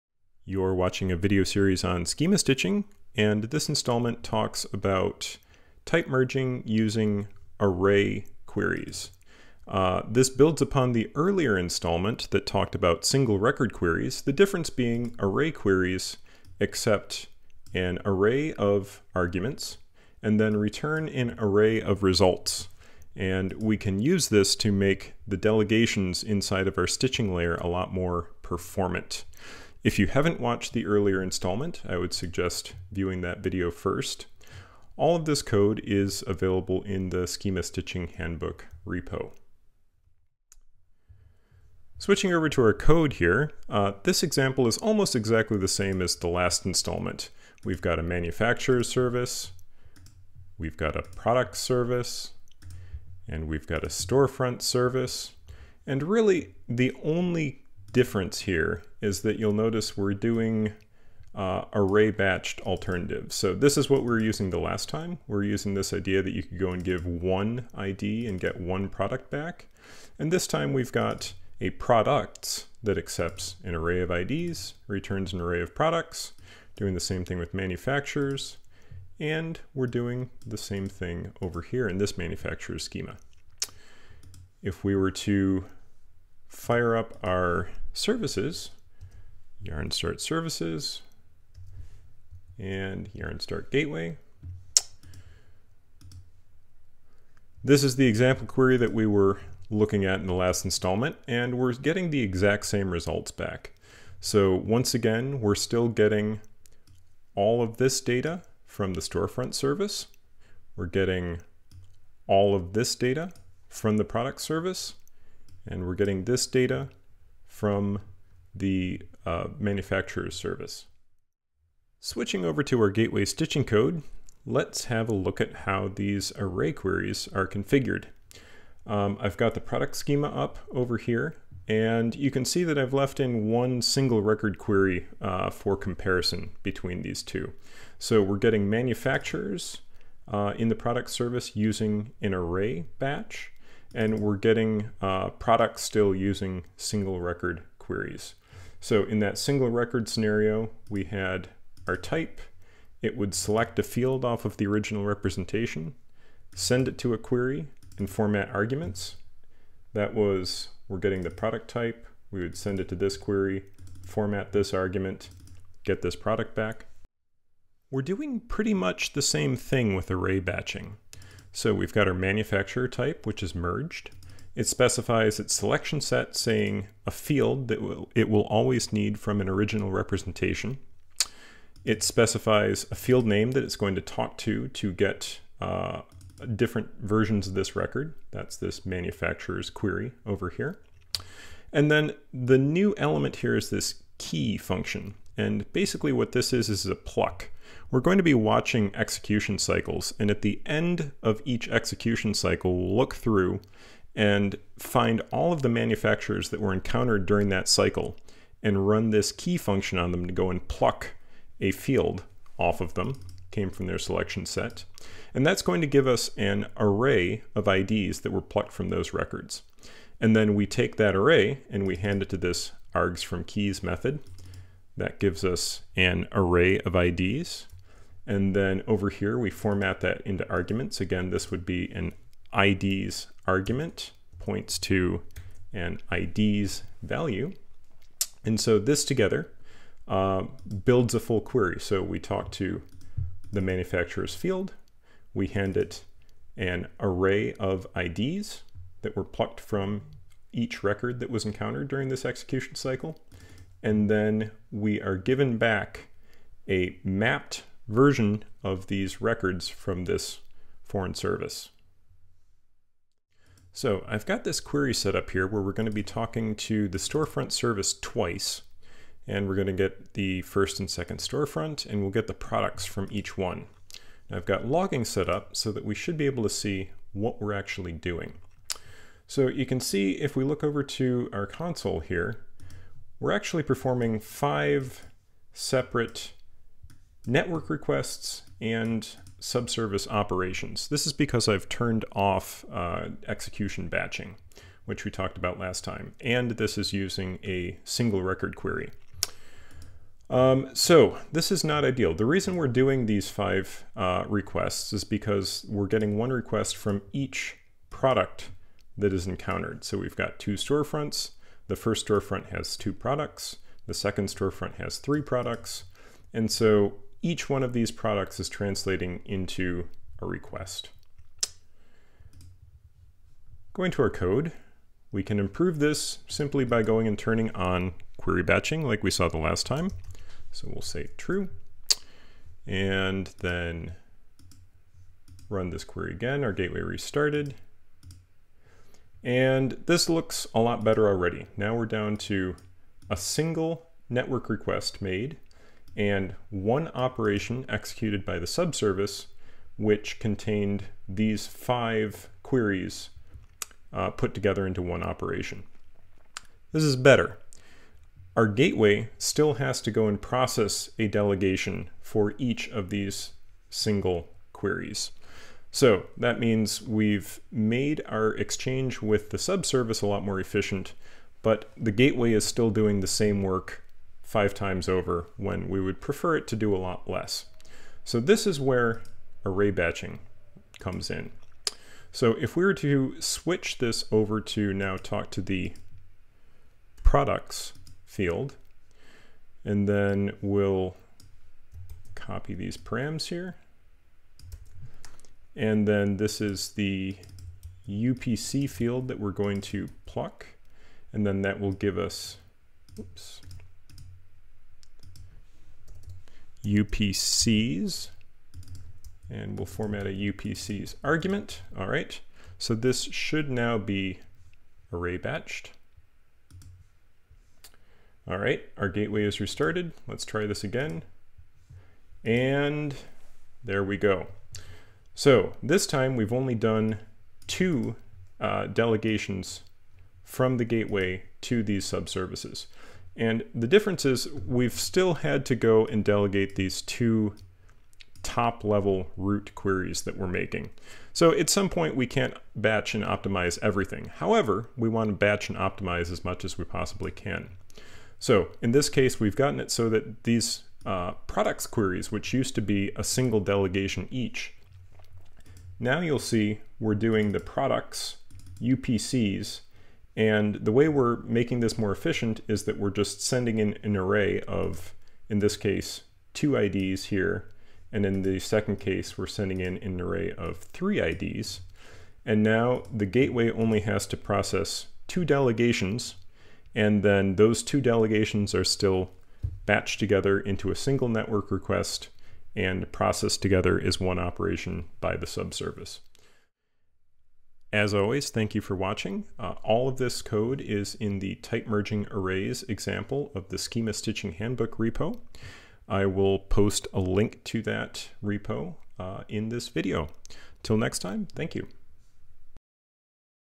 Schema Stitching Handbook – array-batched type merging
This example explores the core techniques for merging typed objects using array queries, covering most of the topics discussed in Batched Type Merging merging documentation.